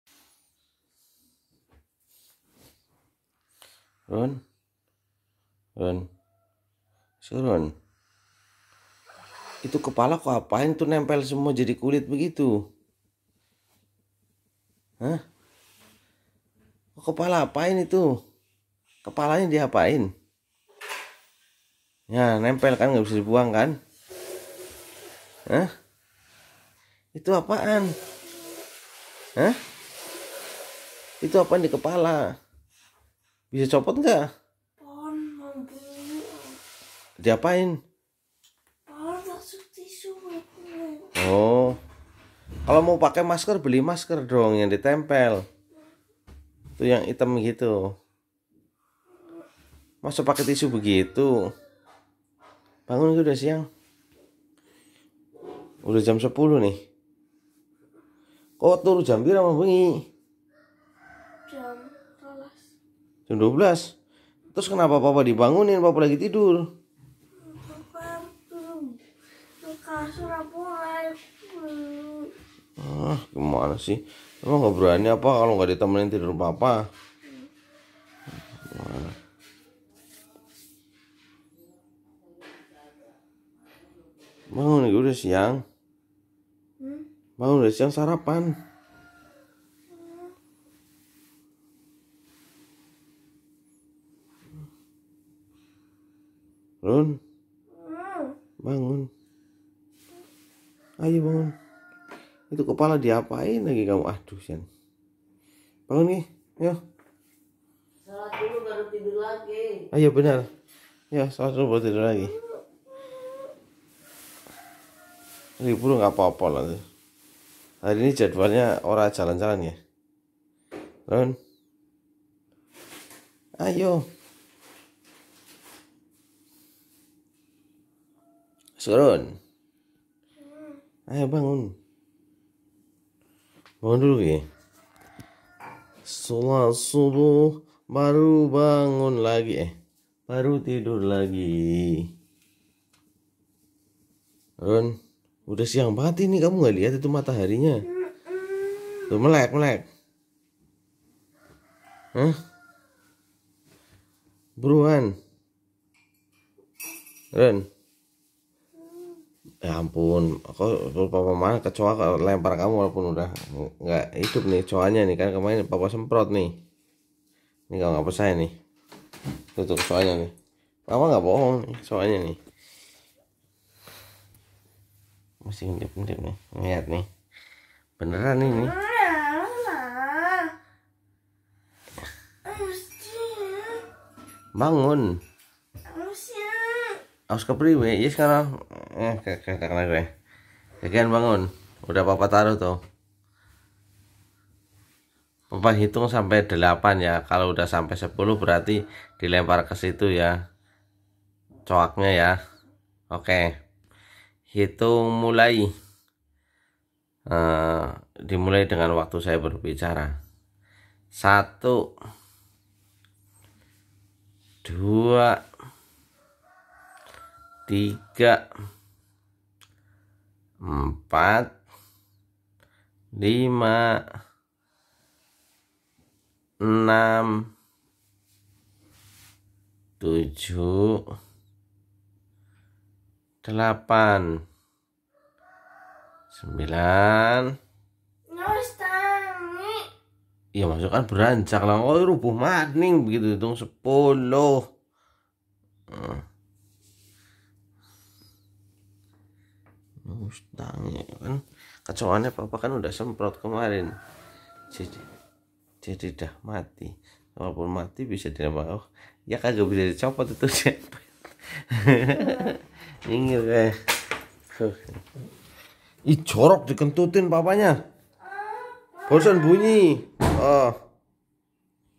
Papa bangunkan putrinya penuh kesabaran kepala tasya bikin papa heran
Papa bangunkan tasya karena udah siang dan ditanya kenapa kepalanya